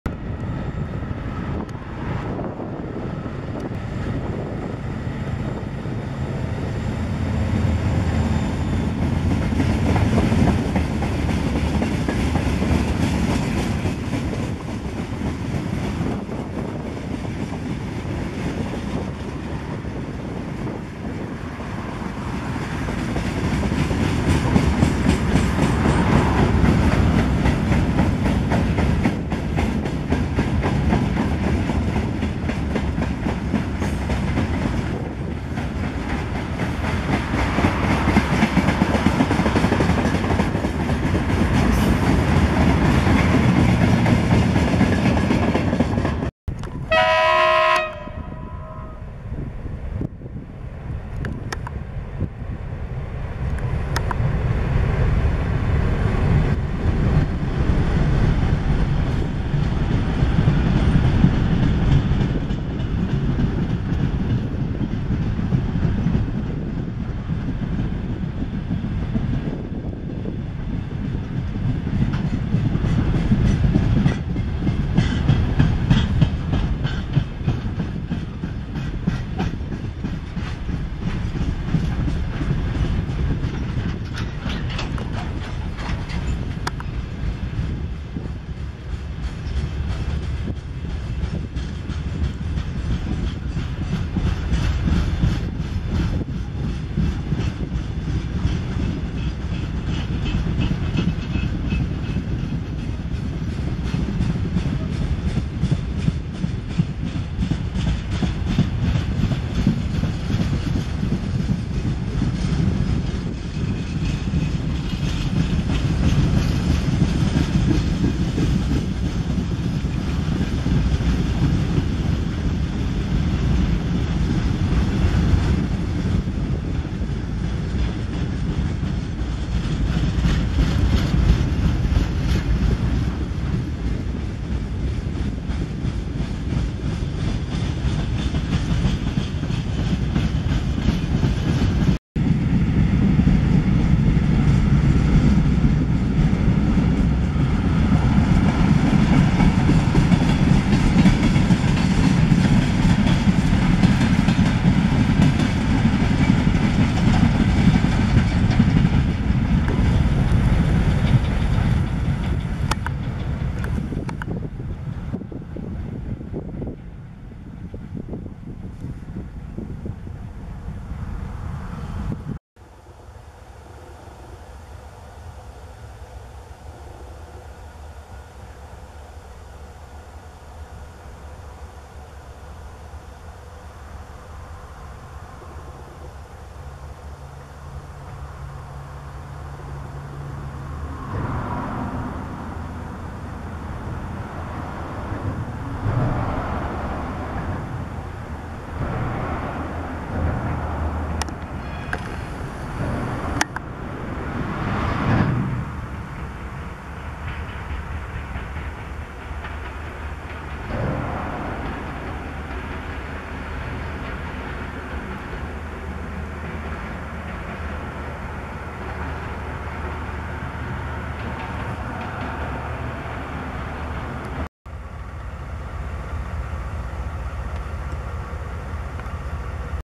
Mieszane szerokie i ST48 008 w Zawadzie
Zawada, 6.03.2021 r. 1) ST40s-008, 2) ST40s-002 i 005, 3) 15D-005 ze zdawką ze Szczebrzeszyna, 4) ST48-008 odczepia się od wahadła simmsów i udaje luzem do Zamościa po 5 simmsów, które zostaną doczepione w Zawadzie do widocznego wahadła z ST48-043, które następnie pojedzie do Bortatycz.